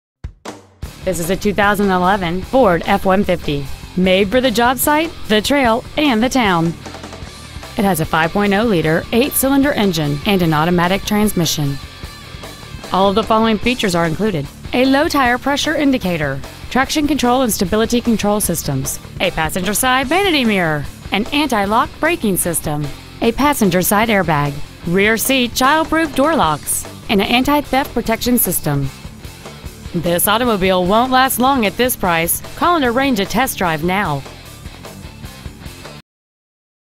2011 Ford F-150 TN
Year : 2011
 Make : Ford
 Model : F-150
 Engine : 5.0L V8
 Trans . : Automatic
 Exterior : Gray

 Interior : Gray
 Marshal Mize Ford